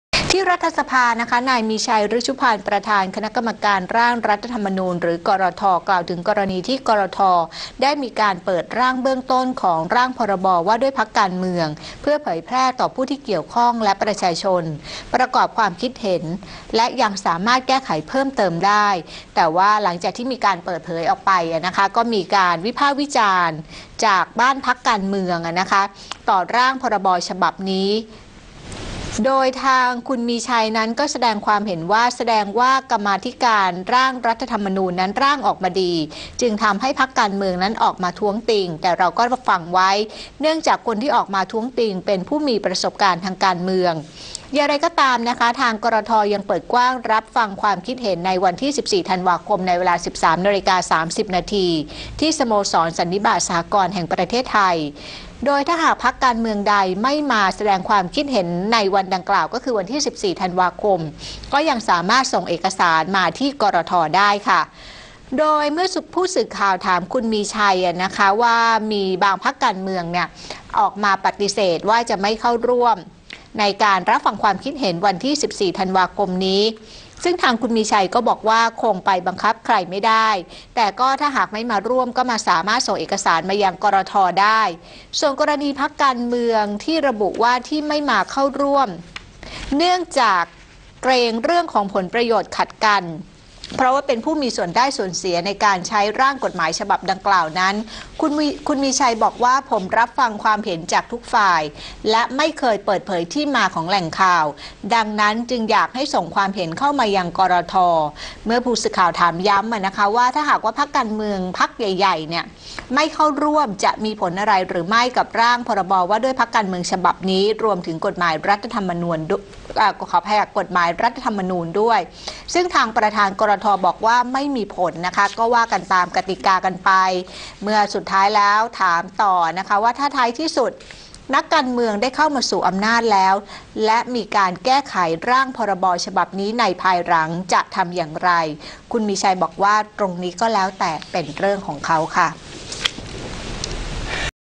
"มีชัย" มองคณะ กรธ.ร่างกฎหมายออกมาดี
2016/12/11 "มีชัย" มองคณะ กรธ.ร่างกฎหมายออกมาดี